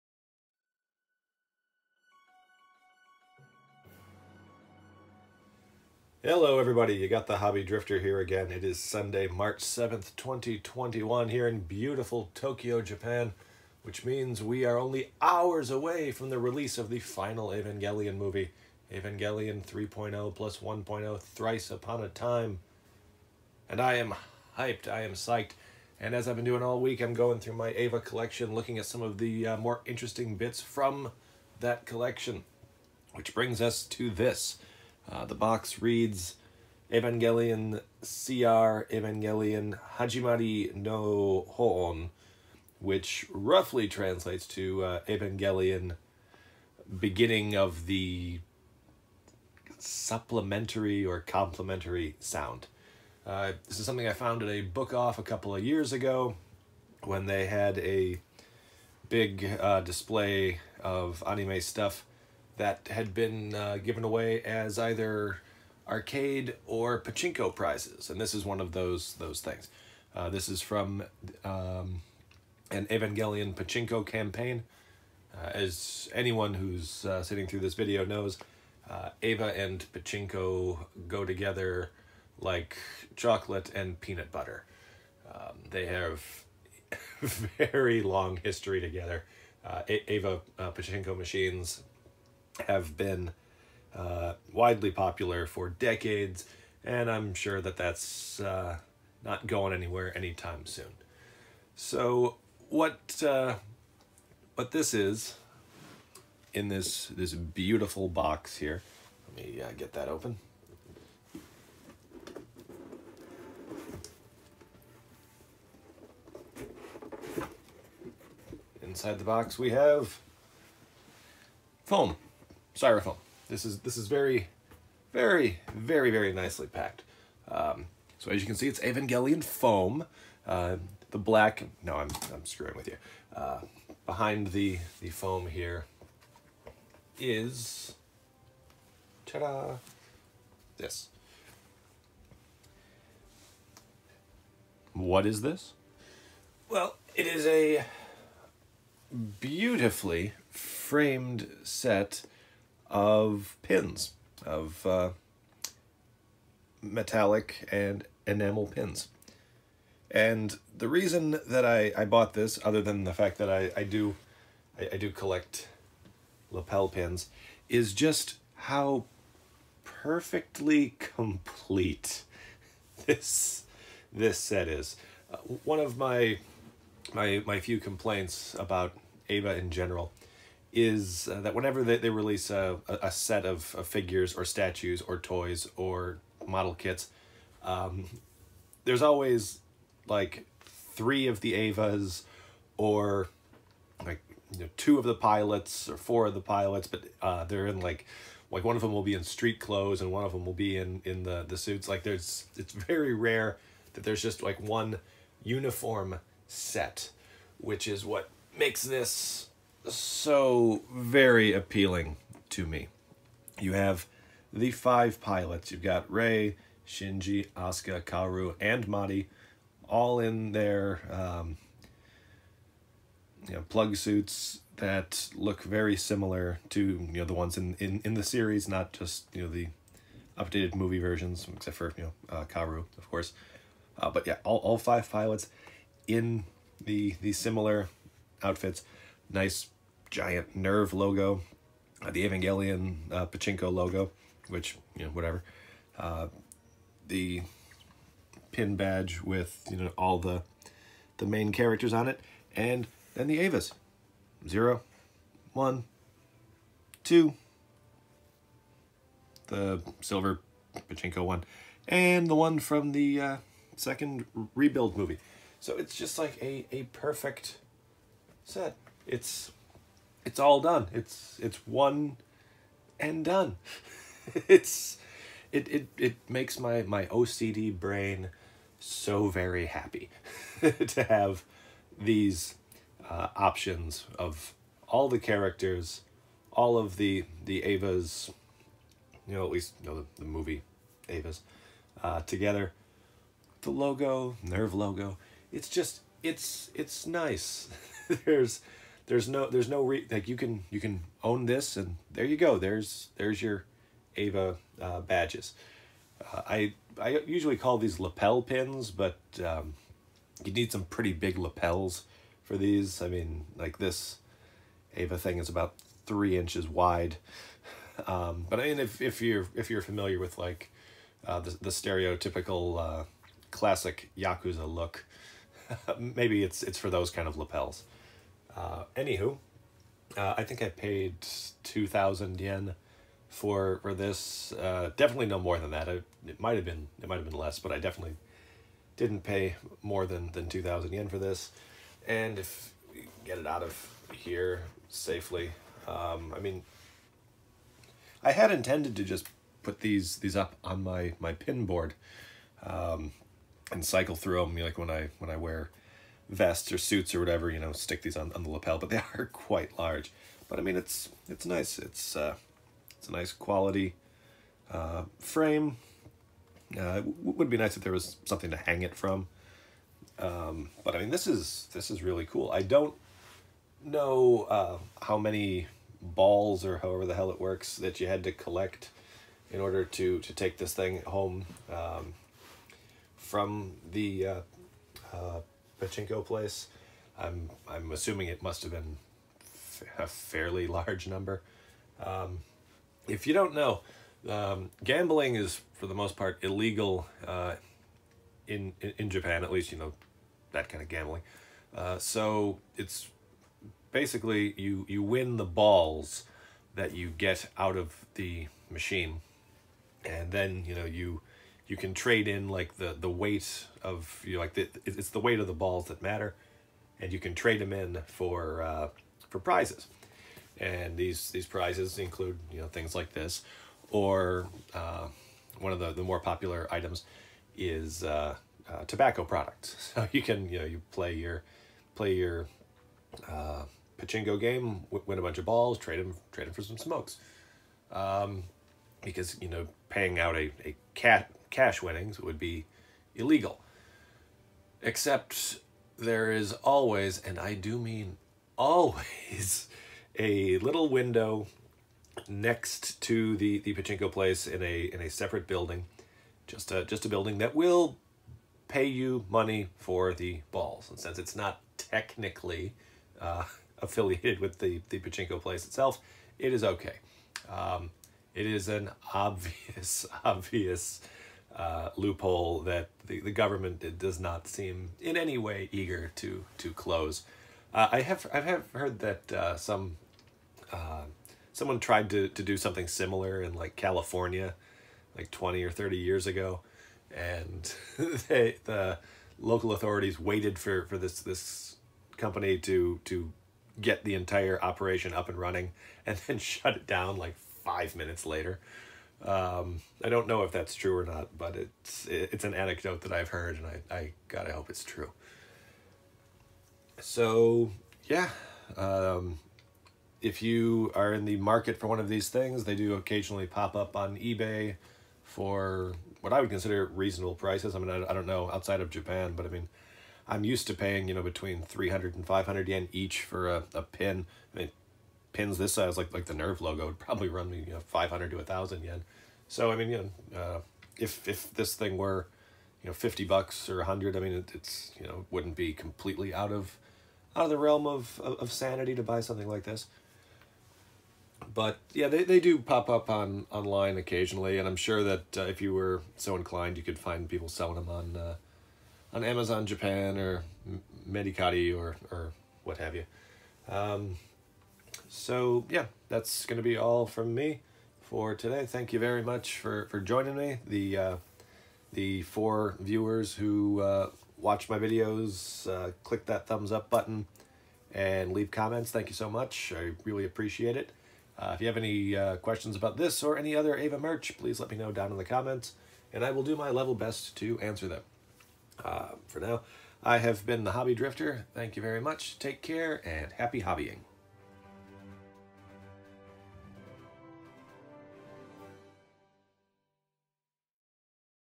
Evangelion Hajimari no Hoonn pachinko pin set
As excellent a set of Evangelion pins as you’re ever likely to see...could only be had by winning a mobbed-up pinball game